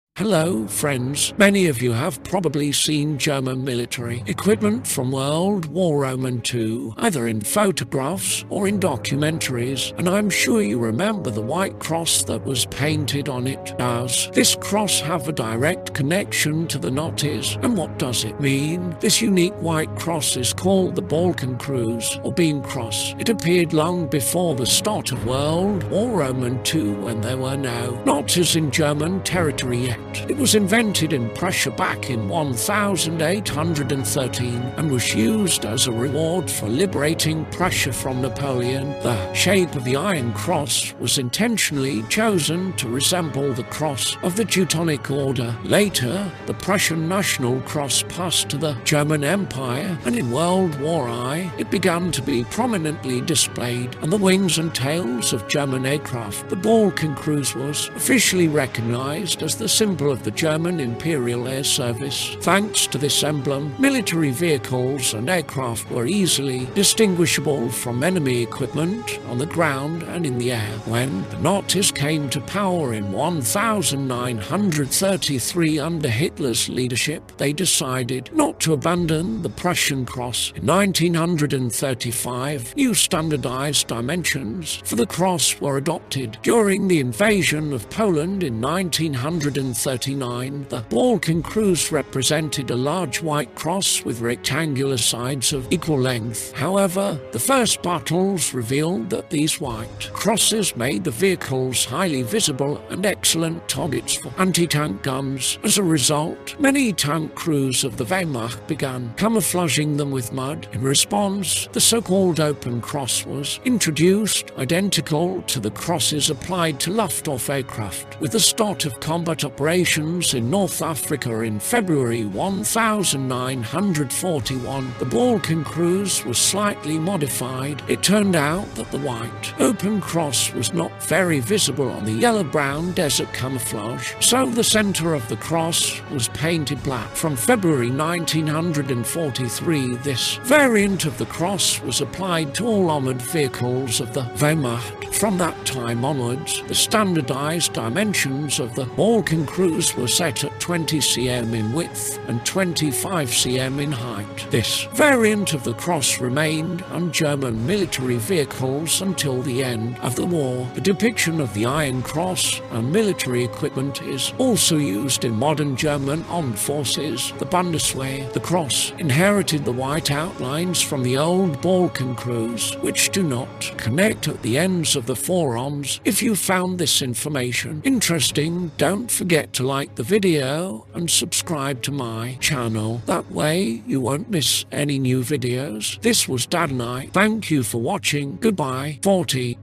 What does the white cross mean on German vehicles in the Wehrmacht and the Bundeswehr I'm sure you
Hello, friends! Many of you have probably seen German military equipment from World War II, either in photographs or in documentaries. And I'm sure you remember the white cross that was painted on it. Does this cross have a direct connection to the Nazis, and what does it mean? This unique white cross is called the Balkenkreuz, or Beam Cross. It appeared long before the start of World War II when there were no Nazis in German territory yet. It was invented in Prussia back in 1813 and was used as a reward for liberating Prussia from Napoleon. The shape of the iron cross was intentionally chosen to resemble the cross of the Teutonic Order. Later, the Prussian national cross passed to the German Empire, and in World War I, it began to be prominently displayed on the wings and tails of German aircraft. The Balkenkreuz was officially recognized as the symbol of the German Imperial Air Service. Thanks to this emblem, military vehicles and aircraft were easily distinguishable from enemy equipment on the ground and in the air. When the Nazis came to power in 1933 under Hitler's leadership, they decided not to abandon the Prussian cross. In 1935, new standardized dimensions for the cross were adopted. During the invasion of Poland in 1939, the Balkenkreuz represented a large white cross with rectangular sides of equal length. However, the first battles revealed that these white crosses made the vehicles highly visible and excellent targets for anti-tank guns. As a result, many tank crews of the Wehrmacht began camouflaging them with mud. In response, the so-called "open cross" was introduced, identical to the crosses applied to Luftwaffe aircraft. With the start of combat operations in North Africa in February 1941, the Balkenkreuz was slightly modified. It turned out that the white open cross was not very visible on the yellow-brown desert camouflage, so the center of the cross was painted black. From February 1943, this variant of the cross was applied to all armored vehicles of the Wehrmacht. From that time onwards, the standardized dimensions of the Balkenkreuz were set at 20 cm in width and 25 cm in height. This variant of the cross remained on German military vehicles until the end of the war. The depiction of the Iron Cross on military equipment is also used in modern German armed forces, the Bundeswehr. The cross inherited the white outlines from the old Balkenkreuz, which do not connect at the ends of the four arms. That way, you won't miss any new videos. This was Daddynight. Goodbye!